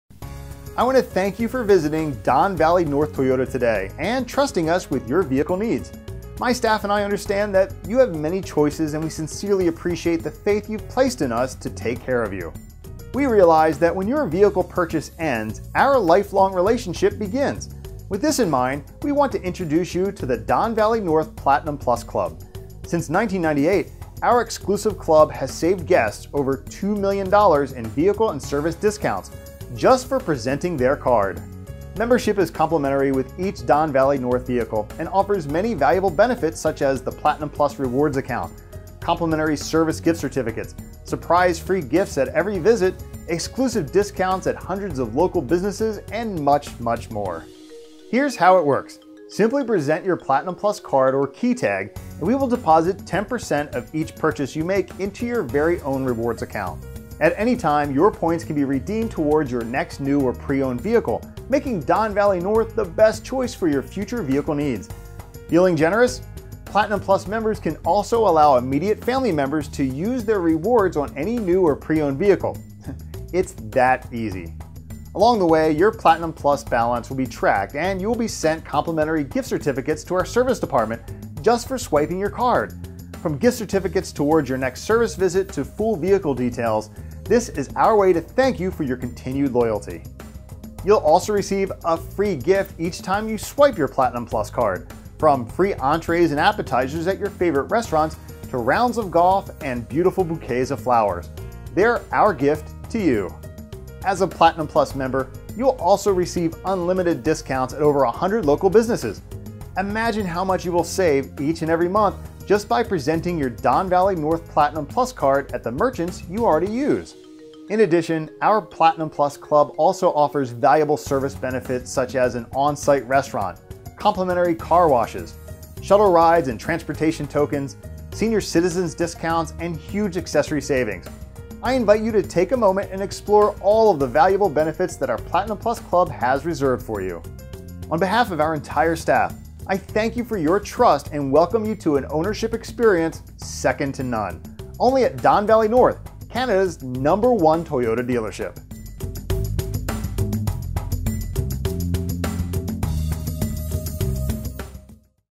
Don Valley North Toyota Platinum Plus
Discover a world of exclusive benefits reserved just for our customers. Swipe your membership card at every visit to earn valuable points toward your next vehicle purchase. Experience VIP treatment in our service department. And, outside of our dealership your rewards card becomes your ticket to savings at your favorite merchants!